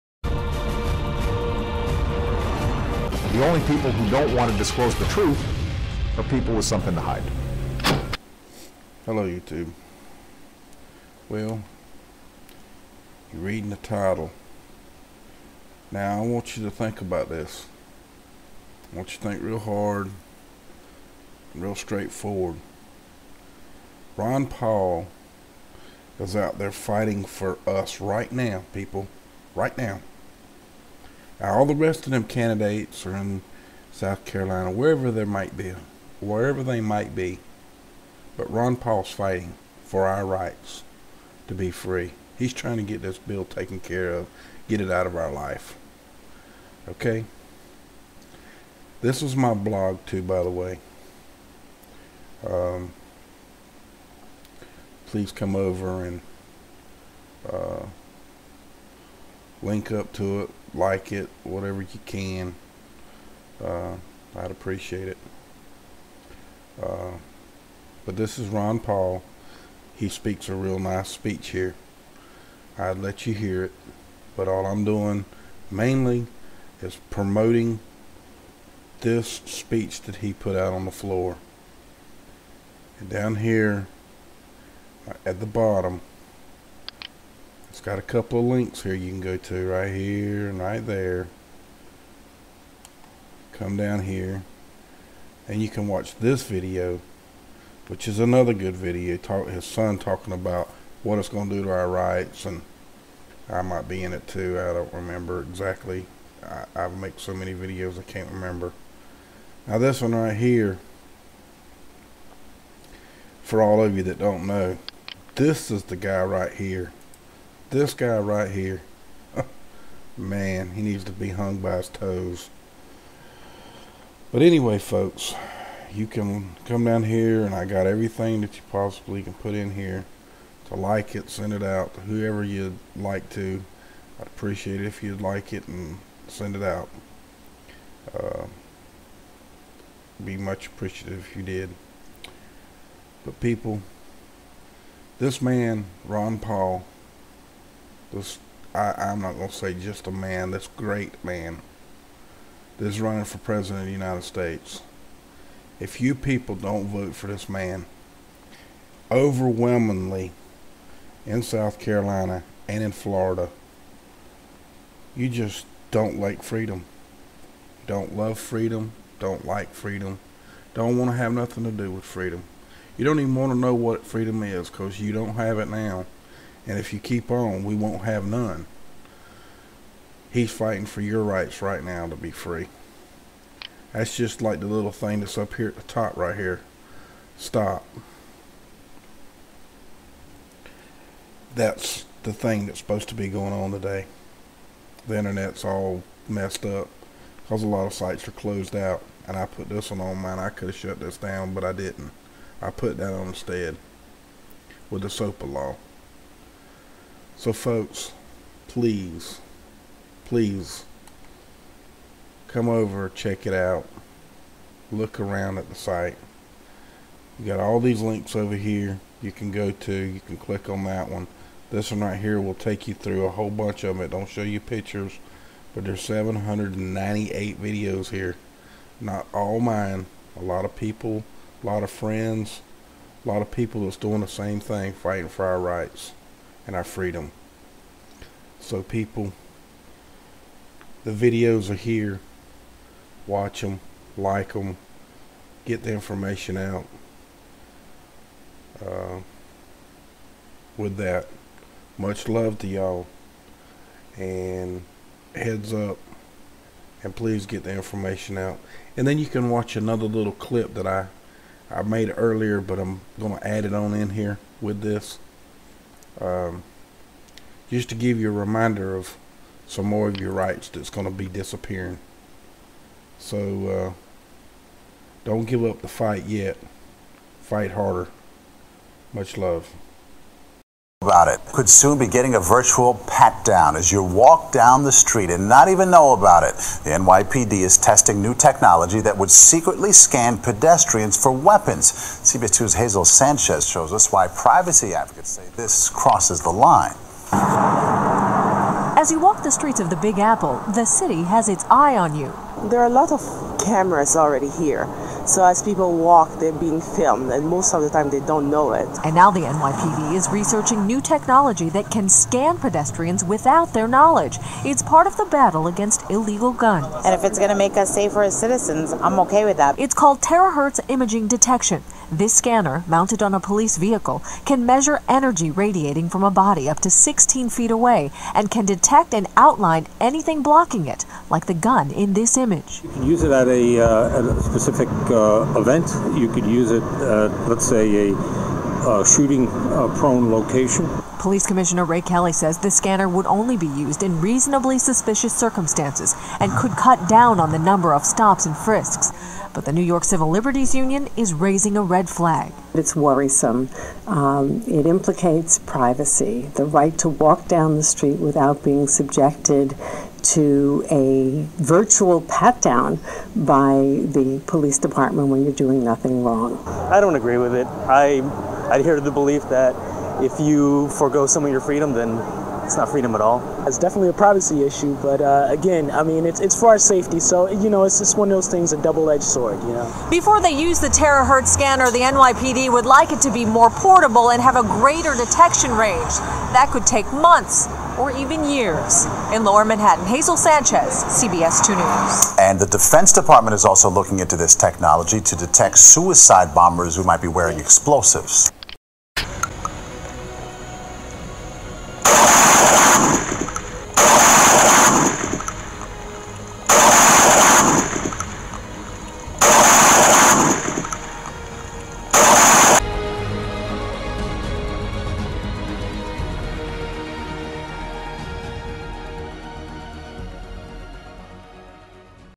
Ron Paul Introduces Legislation to Strike NDAA's Unconstitutional Section 1021.avi
Uploaded by tatoott1009 on Jan 18, 2012